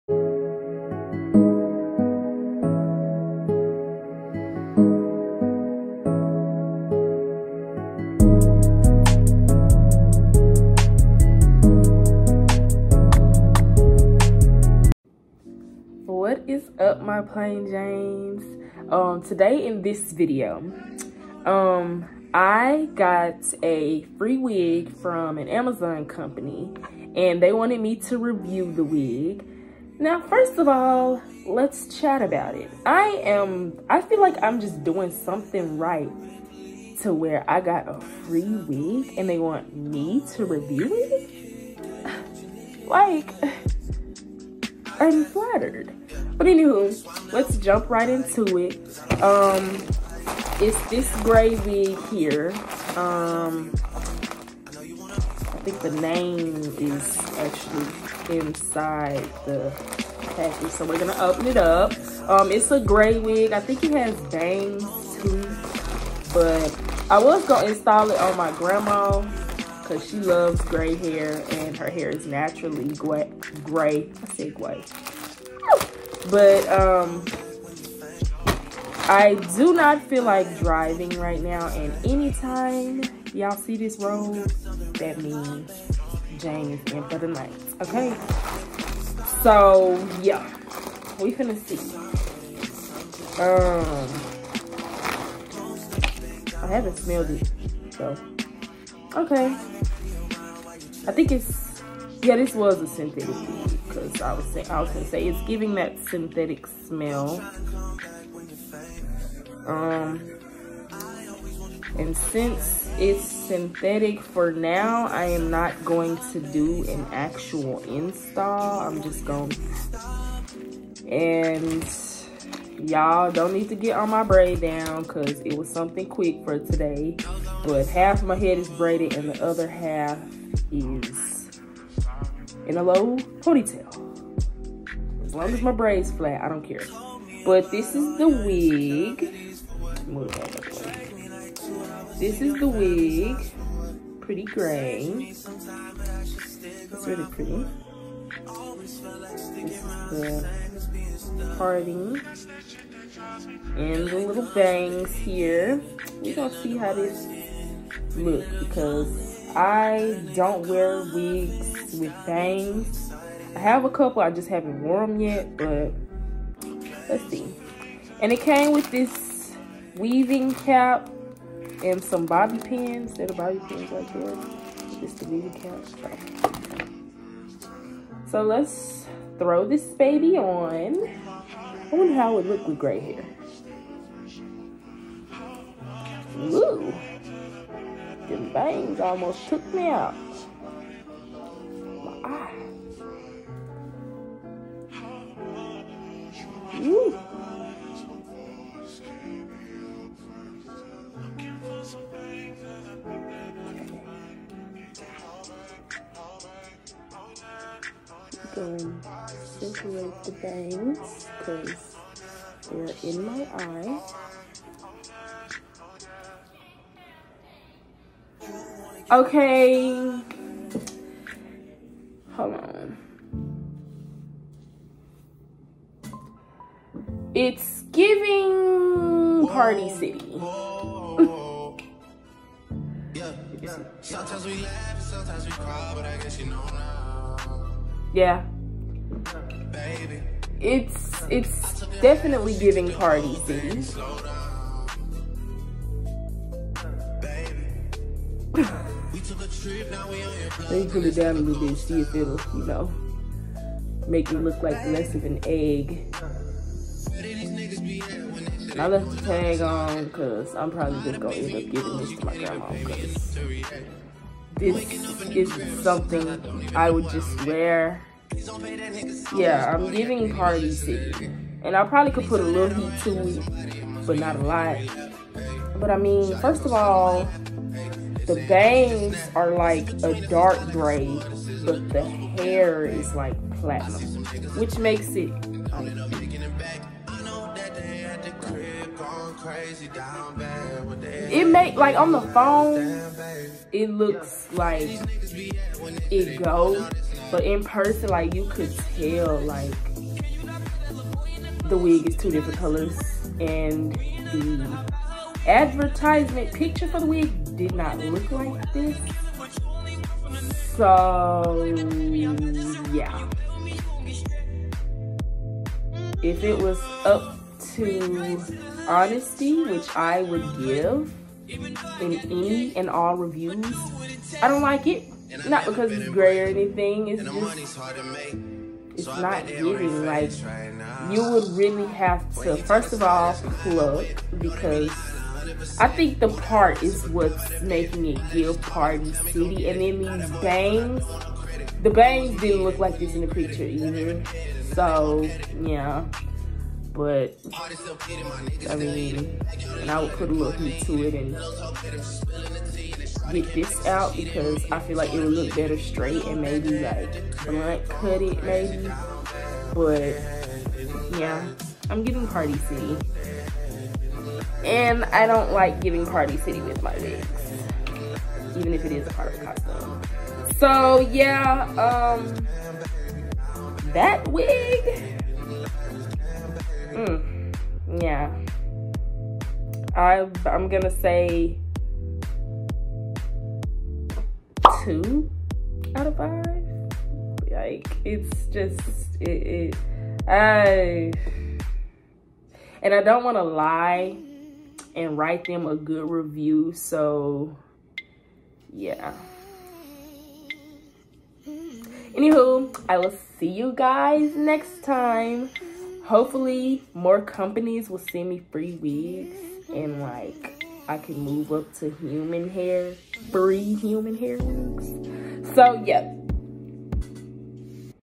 Free Amazon Wig unpackaging
Watch me open up and try on my free Amazon wig I received in exchange for a review